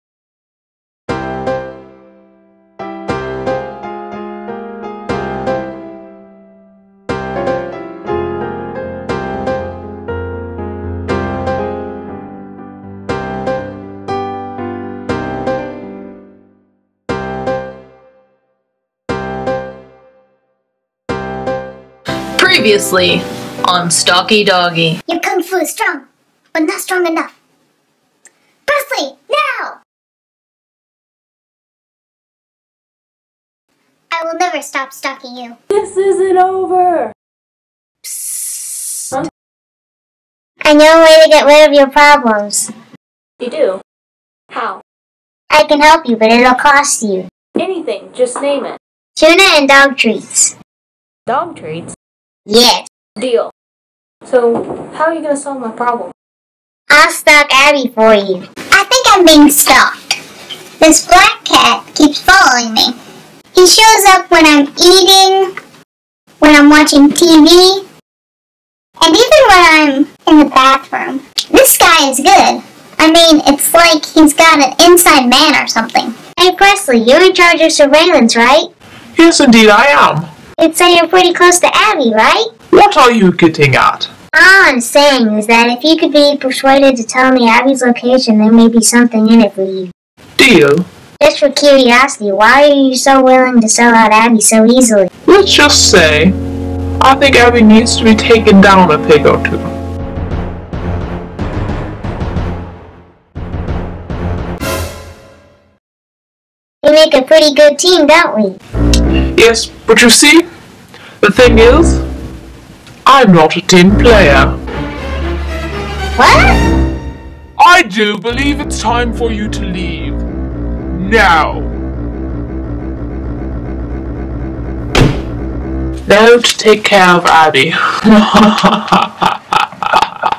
Stalky Doggy Gets Stalked (ep 6 of 9)
This continuation of the Stalky Doggy series features a new character Arthur,the cat,- he is a different cat than the one in Stalky Doggy and Kitty.